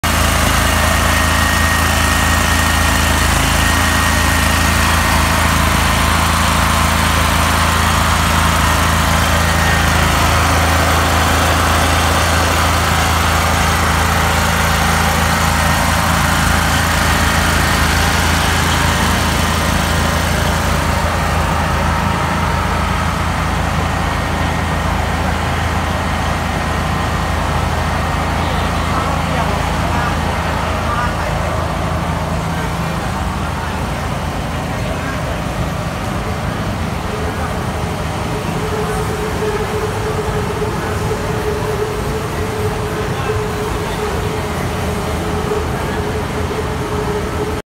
ให้เช่า เครื่องปั่นไฟ เครื่องกำเนิดไฟฟ้า วัดแขก สีลม
ให้เช่าเครื่องปั่นไฟ Generator พิธีนวราตรี และงานแห่ประเพณี เจ้าแม่อุมาเทวี วัดพระศรีมหาอุมาเทวี วัดแขก สีลม ถนนสีลม เขตบางรัก กรุงเทพมหานคร
Single phase Gasoline Generator Set
ANATA AG-6500EA Rate Voltage 220V Rate Frequency 50Hz Rate output 5KW,Max5.5KW Power factor 1.0
T.0815778766